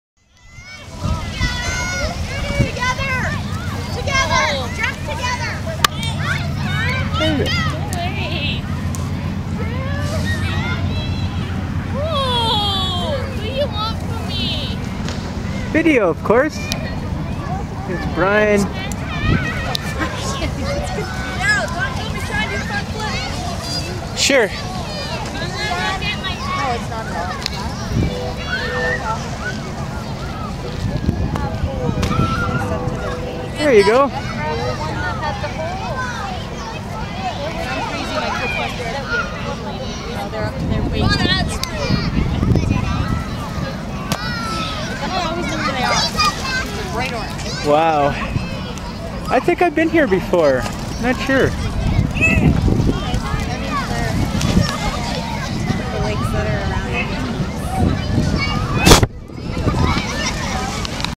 Old Home Video - 2011 08 22 (low quality wmv)
Camping with my daughter, Raven. And my wife's son, Bryan.